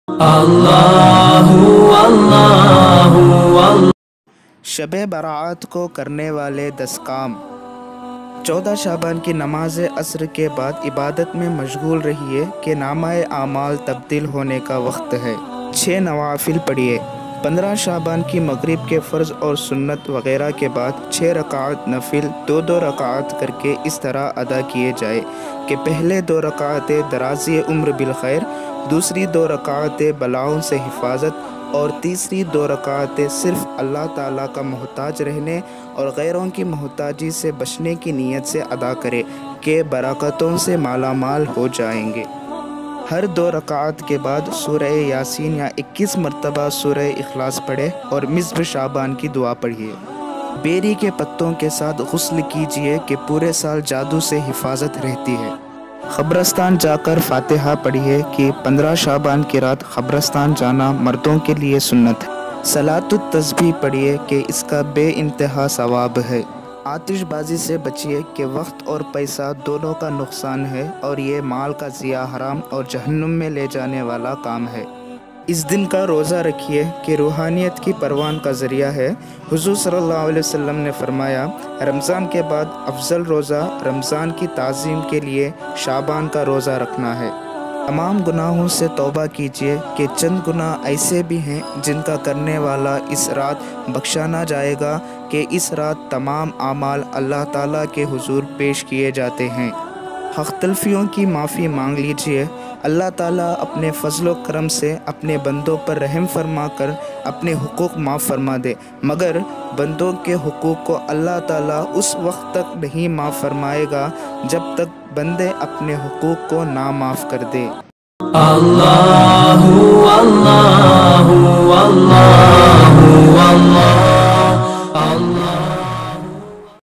shab e barat ko ye 10 kaam zaroor kare||10 things to do on shab e barat(hindi)
Assalam alaikum mere pyare Islami bhaiyon
Shab e barat ko karne waale 10 kaam
Ise zaroor amal kare

Kuch galtiyan ho toh comment me zaroor bataye

Topics
1.shab e barat ki Haqeeqat.
2.shab e barat ki fazilat.
3.shab e barat ko karne waale 10 kaam.
4.Shab e barat ko kya kare